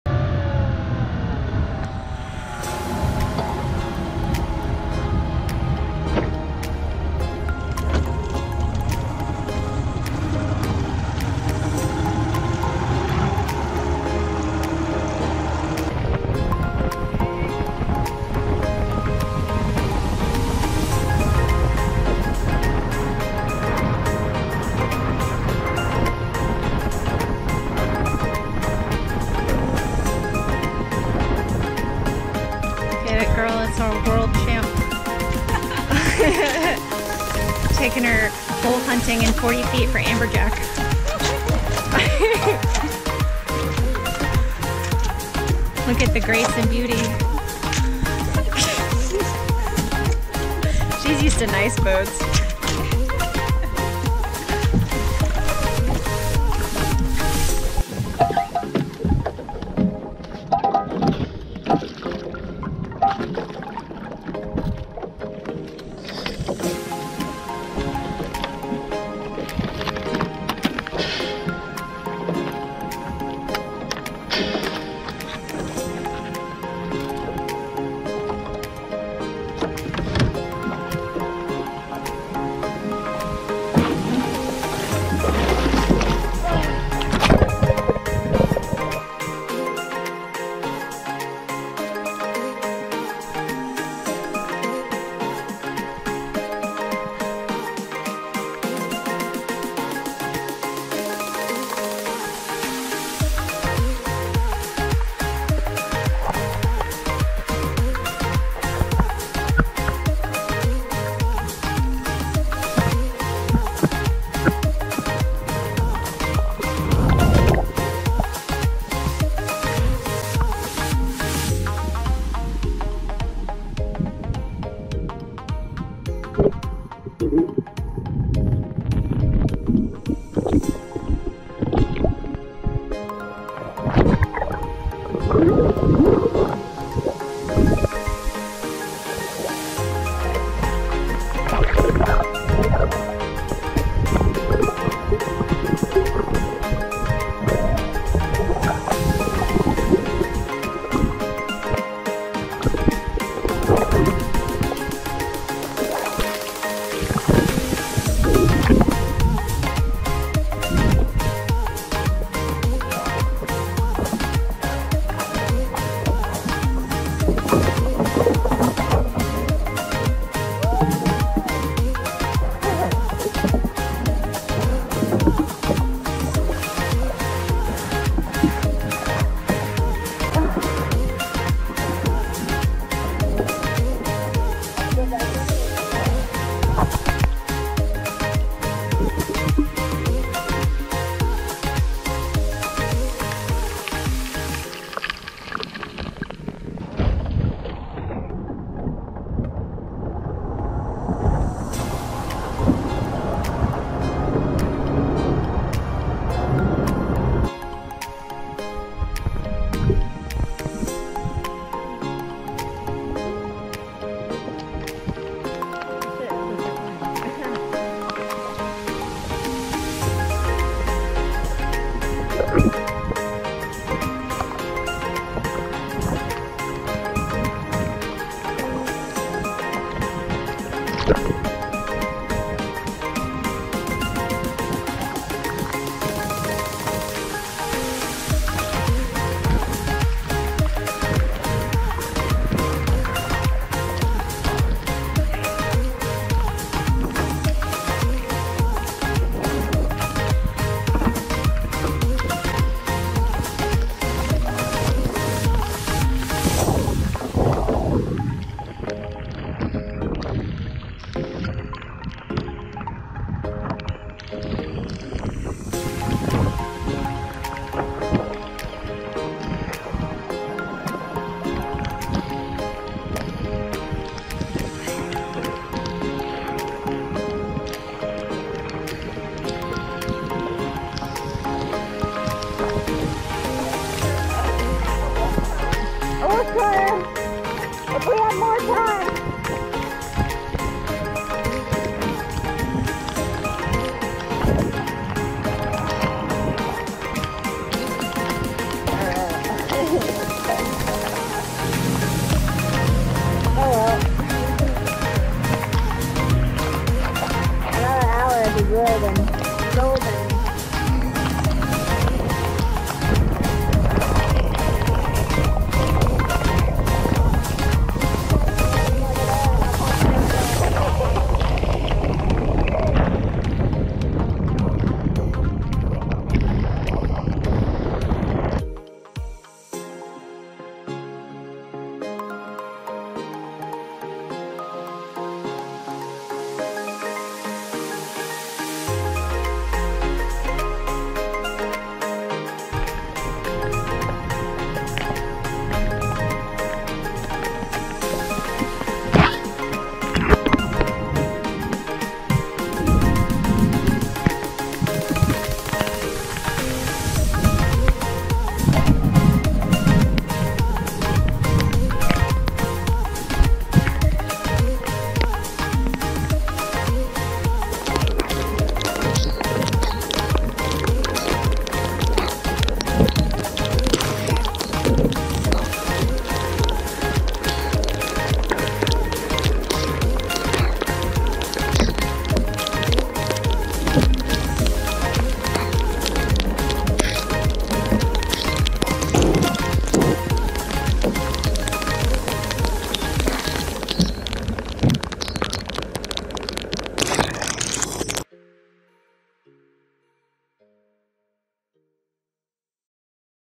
Girls playing in the rain. Skiff Spearfishing.
Took Nichole Burko out on my skiff spearfishing. We got a late start and got chased in early by thunderstorms. We tried to hit the river but it was too dirty. It rained so hard my boat almost completely flooded.

© Licence ▼

Limujii:
   / limujii

📨 Track Submission ▼